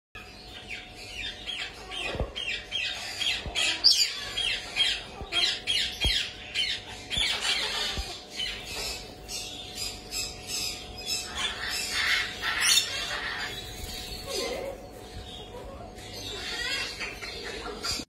Male Blue Throated Conure | Victorian Bird Co
This beautiful Male Blue Throated Conure just arrived. Pop down for a cuddle at our Hoppers Crossing store.

2/2 Shaft Court Hoppers Crossing VIC 3029
57 Alkira Ave, NORLANE VIC 3214